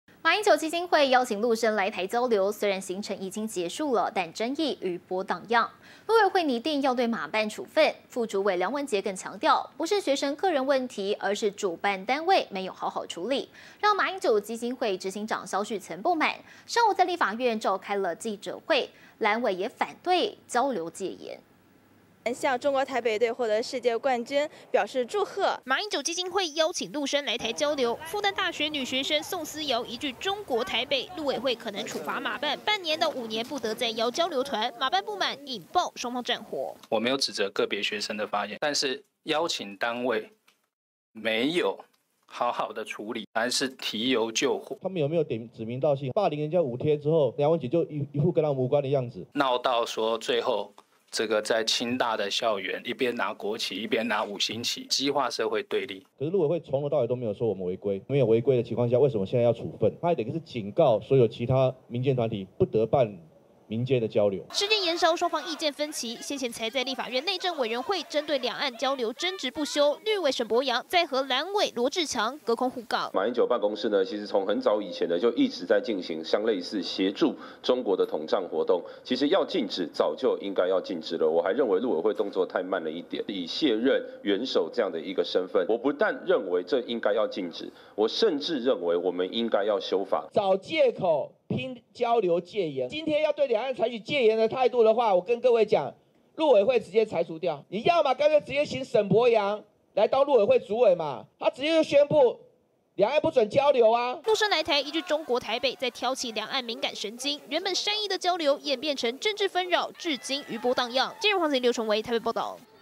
陸委會擬處分馬辦 蕭旭岑反控霸凌中生
馬英九基金會，邀請陸生來台交流，雖然行程已結束，但爭議餘波盪漾，陸委會擬定要對馬辦處分，副主委梁文傑更強調，「不是學生個人問題，而是主辦單位沒有好好處理」，讓馬英九基金會執行長蕭旭岑不滿，(周五)上午在立法院開記者會，藍委直呼是「交流戒嚴」，綠委則是力挺陸委會的做法，甚至認為應該要要修法規範。